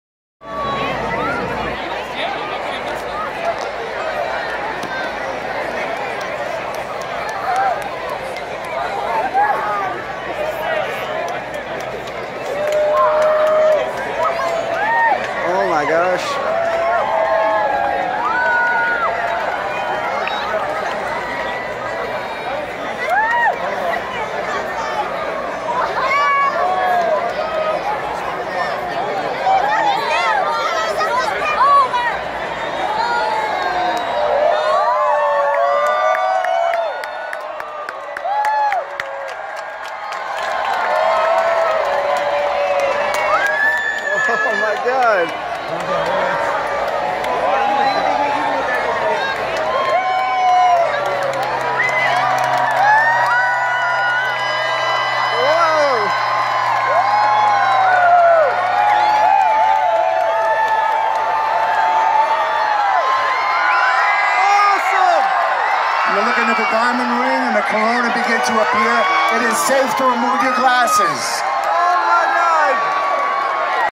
2017 Eclipse Over Clemson, SC - August 21, 2017 - Beginning of Totality
Sorry for the shakiness and screaming. The experience was very moving to say the least!